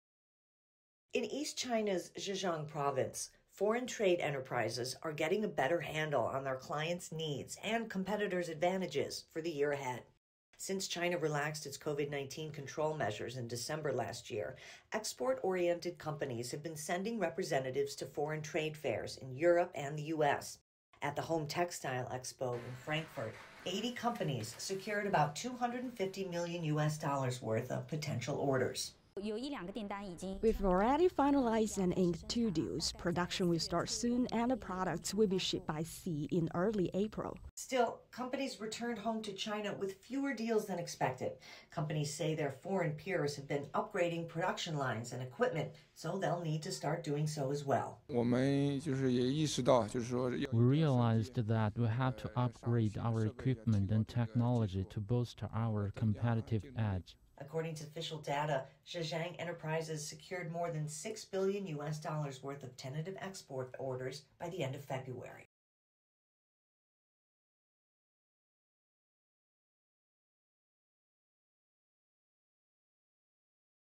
Foreign trade companies in east China rushing to fill orders
n east China's Zhejiang Province, foreign trade enterprises are getting a better handle on their clients' needs, and competitors' advantages, for the year ahead.

Since China relaxed its COVID-19 control measures in December last year, export-oriented companies have been sending representatives to foreign trade fairs in Europe and the U.S.

At the home textile expo in Frankfurt, 80 companies secured about 250 million U.S. dollars-worth of potential orders.

Still, companies returned home to China with fewer deals than expected.

Companies say their foreign peers have been upgrading production lines and equipment, so they’ll need to start doing so as well.

According to the city's official data, Zhejiang enterprises secured more than 6 billion U.S. dollars-worth of tentative export orders by the end of February.